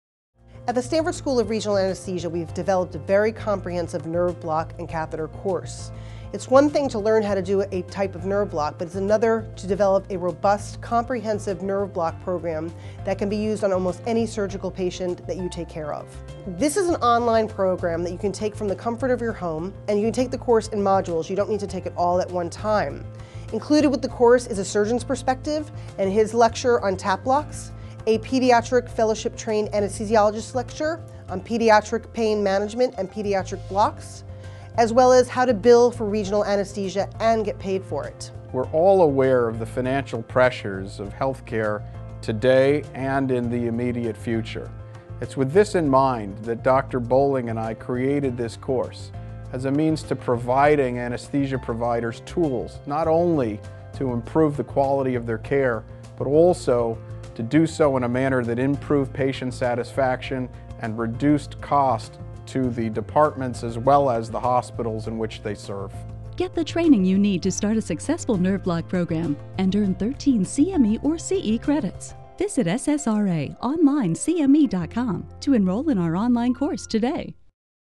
SSRA Online Anesthesia Training Program - SSRAUSA.com
Visit SSRAUSA.com to register for the next Ultrasound Guided Regional Anesthesia Seminar.

Course Modules

How to Develop a Robust Nerve Block Program and Overcome the Common Obstacles Created by Your Partners, Surgeons, Administrators, and Nurses

1. Nerve block program development
2. Learn how to overcome common obstacles
3. Understand HCHAPS scores and how they will affect pain management

A Surgeon's Perspective of Why Regional Anesthesia is Good For the Surgeon

1. Why regional anesthesia is good for your surgeon
2. How to get "buy-in" from the surgeon.
3. Why TAP blocks are great for hernia repairs

Billing For Regional Anesthesia and Getting Paid For It

1. Coding for regional anesthesia
2. Tips for negotiating payment for regional anesthesia
3. Billing for regional anesthesia

Upper Extremity Blocks

1. Techniques for interscalene, supraclavicular, and infraclavicular single shot blocks and catheters
2. Anatomy of the brachial plexus
3. Ultrasound imaging of the brachial plexus
4. Indications, contraindications, and literature review for upper extremity blocks
5. Understanding the use of brachial plexus catheters in the outpatient setting
6. Video tutorial of ultrasound imaging of the brachial plexus

Lower Extremity Blocks

1. Techniques for femoral, fascia iliaca, sciatic (posterior and anterior approach), saphenous, and popliteal single shot blocks and catheters
2. Anatomy of the nerves in the lower extremity
3. Ultrasound imaging of the nerve in the lower extremity
4. Indications, contraindications, and literature review for lower extremity blocks
5. Understanding the use of lower extremity blocks in an outpatient setting
6. Video tutorial of ultrasound imaging of the nerves in the lower extremity

Truncal Blocks: TAP and Paravertebral Blocks (PVB)

1. Techniques to perform TAP blocks (mid-axillary and sub-costal approaches) and thoracic paravertebral blocks
2. Anatomy, sonoanatomy, indications, contraindications, and literature review for TAP and PVB single shot blocks and catheters
3. Video tutorial of ultrasound imaging of TAP and PVB

The Role of a Dedicated Block Nurse in a Successful Regional Anesthesia Program

1. Understand the role of a block nurse in a regional anesthesia program
2. Pre-block, assisting with the block, and post-block roles

Pediatric Regional Anesthesia

1. The history of pediatric regional anesthesia
2. Anesthesia and learning disabilities
3. Advantages of regional anesthesia in a pediatric population
4. Anatomic, physiologic, pharmacologic, and psychiatric differences of pediatric patients compared to adults